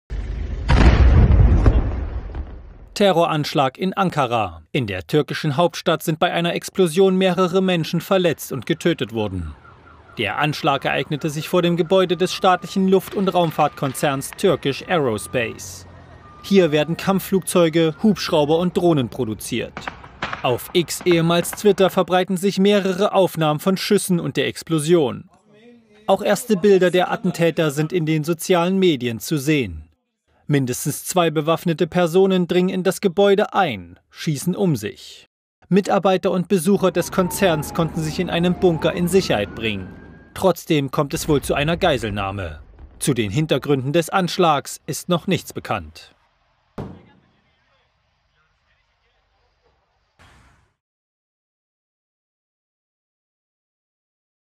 Terror in Ankara (Türkei) - Tödlicher Anschlag:
Terroristen stürmen einen Rüstungskonzern (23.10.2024).
Bisher 5 Tote & 22 Verletzte.
(Quellen: X / BILD).